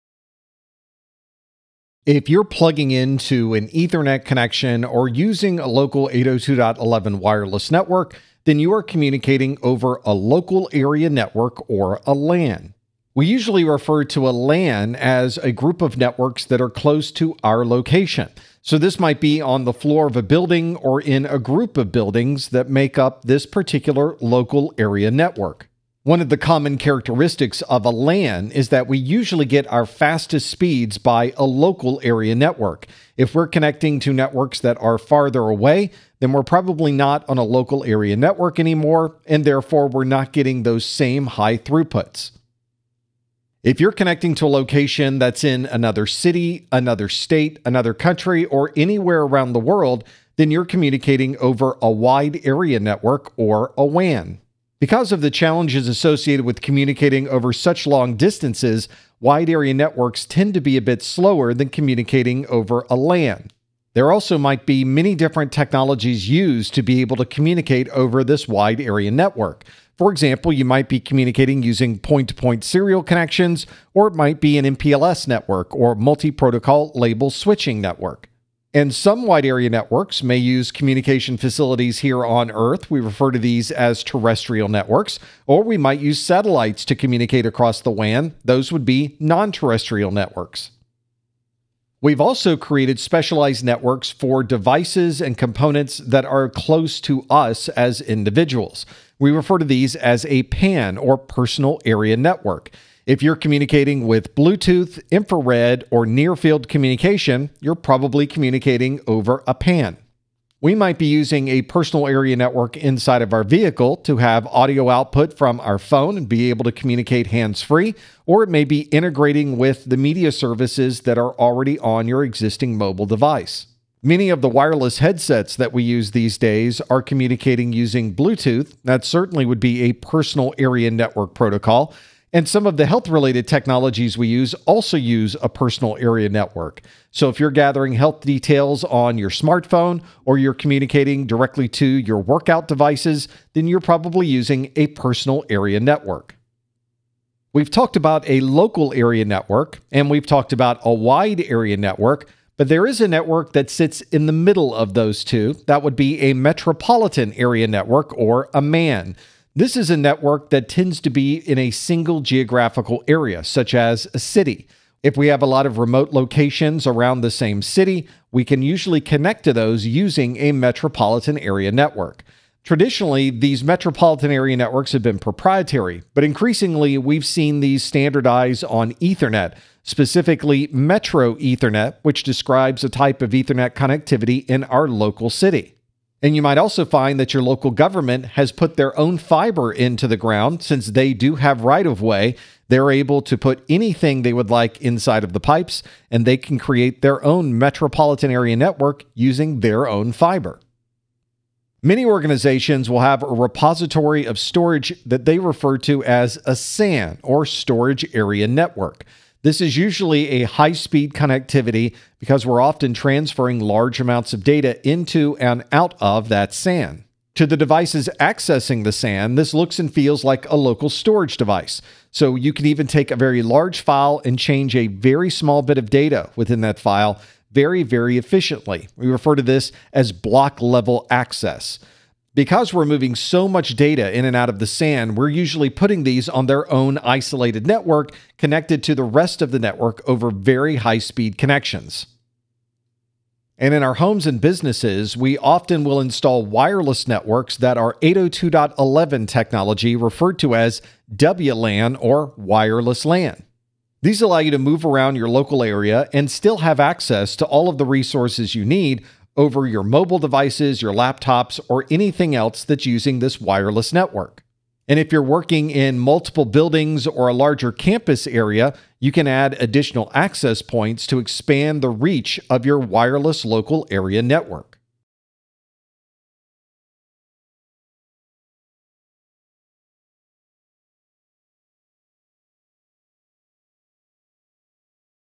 Network Types - CompTIA A+ 220-1201 - 2.7
The categorization of a network is closely associated with its size and location. In this video, you’ll learn about LANs, WANs, PANs, SANs, and WLANs.